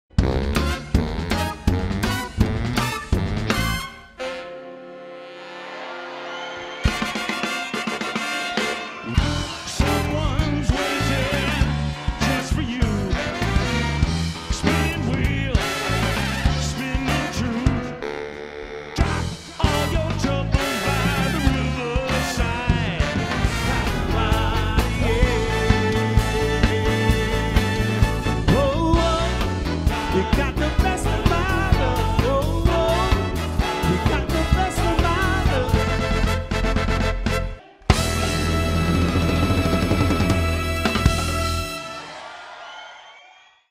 Jazz @ LBP Hosted by Jaymz Bee featuring OAKLAND STROKE (Trailer)
Featuring music by Tower of Power, Earth Wind & Fire, Blood Sweat & Tears, Chicago, and more, JAZZ @ LBP featuring OAKLAND STROKE is going to blow the roof off LBP Brampton on April 11, hosted by Jaymz Bee!